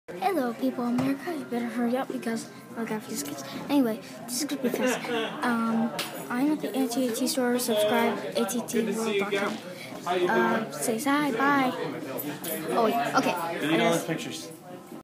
AT&T store video
Kid leaves video on the stores iPad while his parents shop.